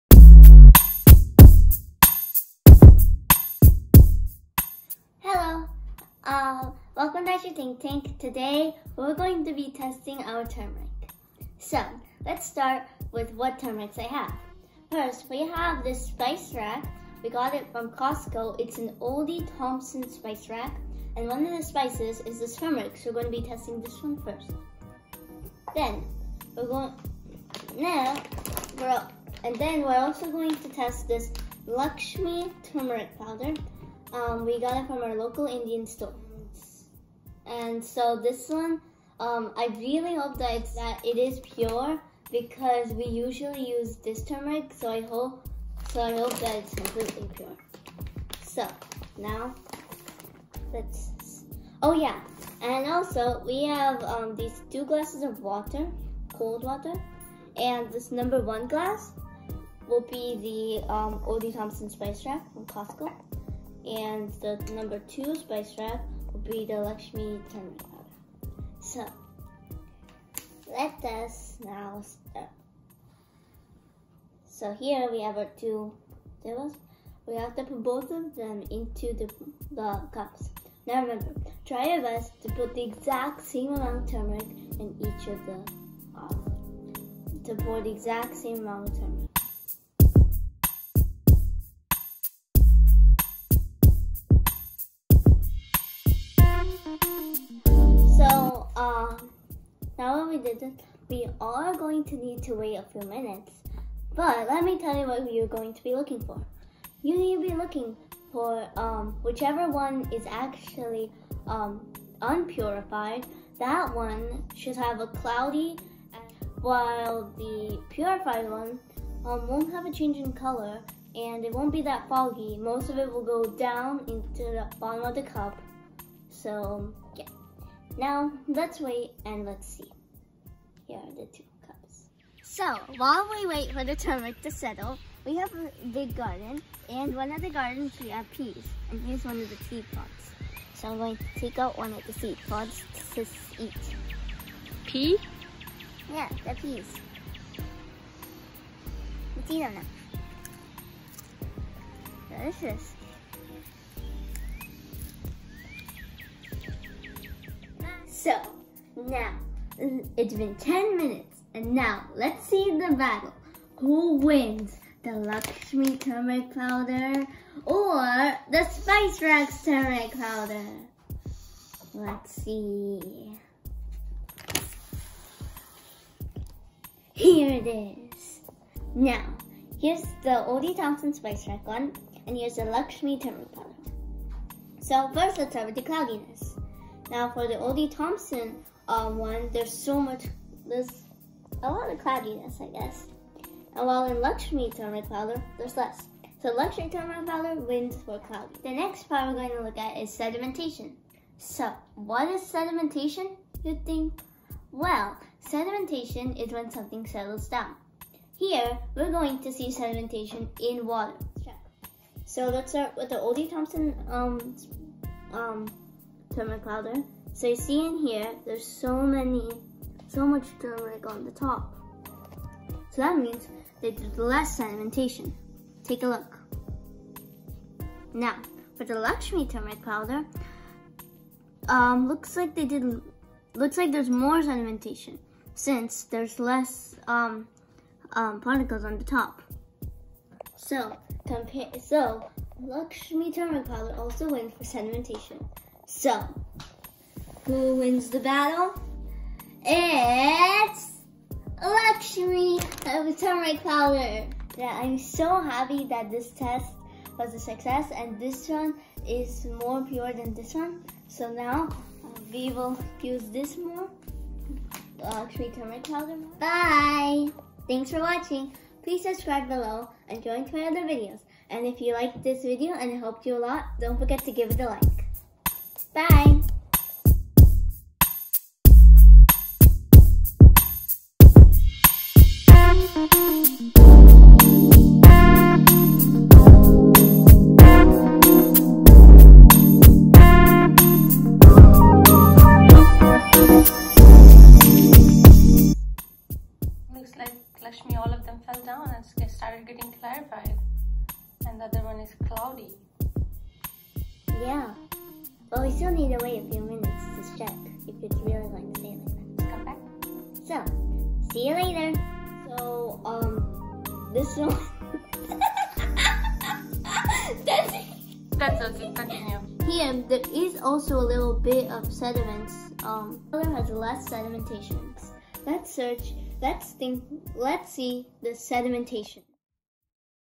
Turmeric Purity Testing
Simple home testing for finding turmeric adulteration